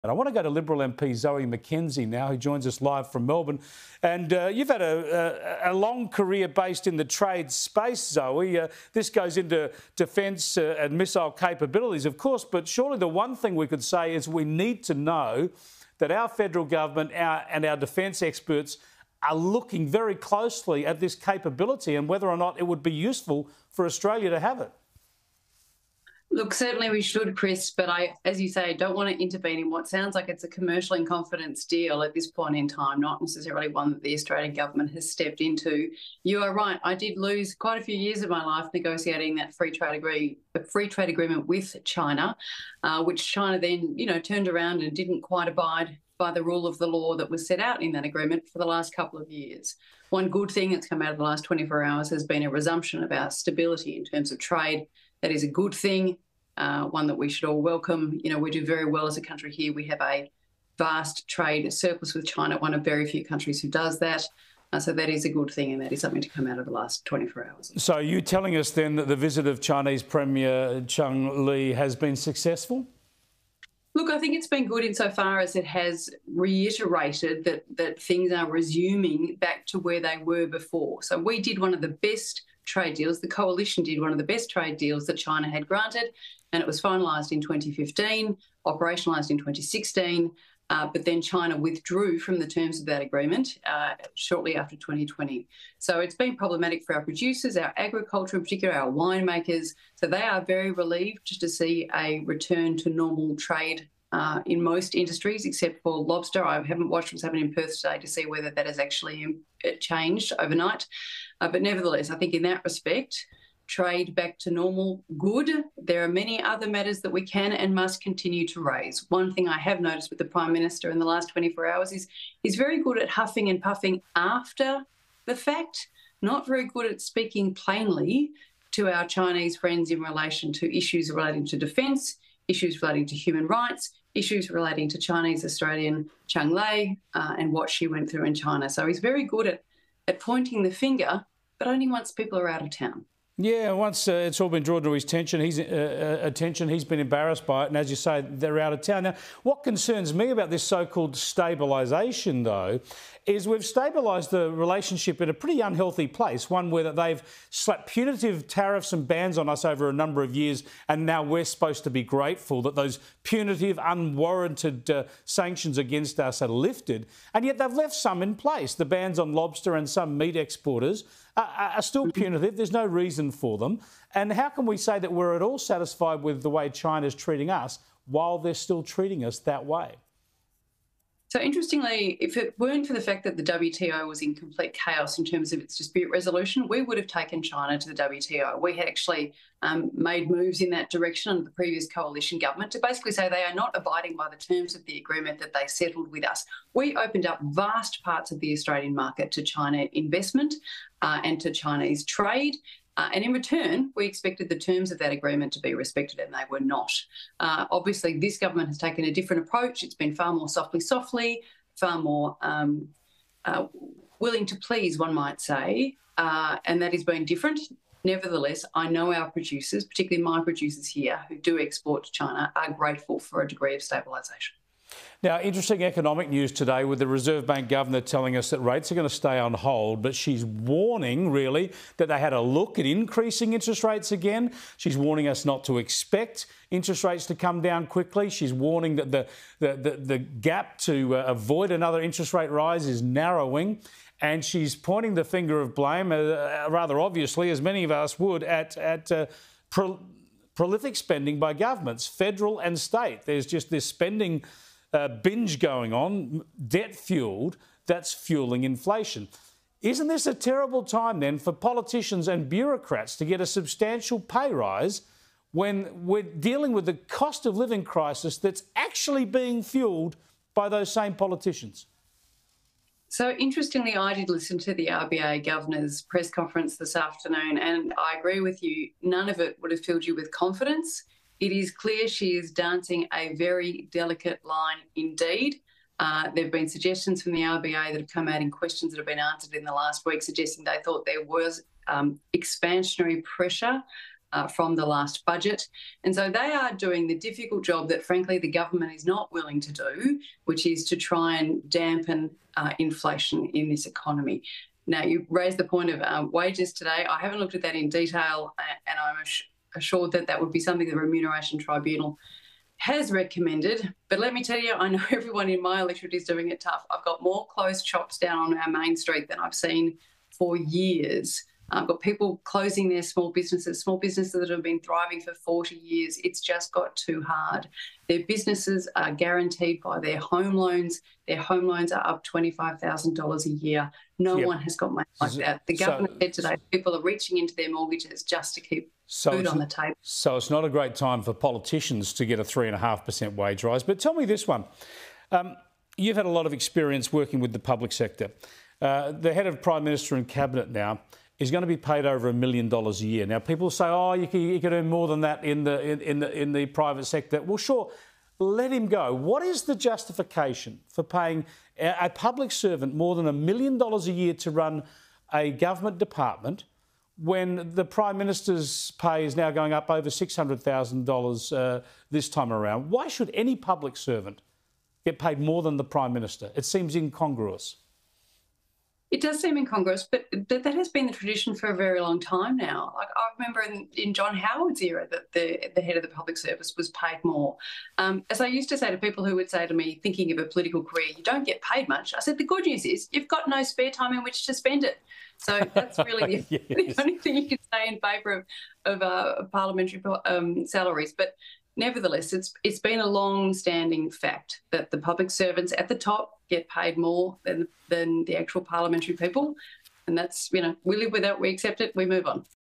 Defence experts look ‘very closely’ at missile manufacturer potentially falling to China
Labor is facing accusations it is sitting idly by as China swoops in to buy a major South American missile manufacturer which could fall into Australian hands if ministers act quickly.

Sky News host Chris Kenny says defence experts are looking “very closely” into the matter.

“This goes into defence and missile capabilities of course but surely the one thing we could say is we need to know that our federal government and our defence experts are looking very closely at this capability,” Mr Kenny said.

“And whether or not it would be useful for Australia to have it.”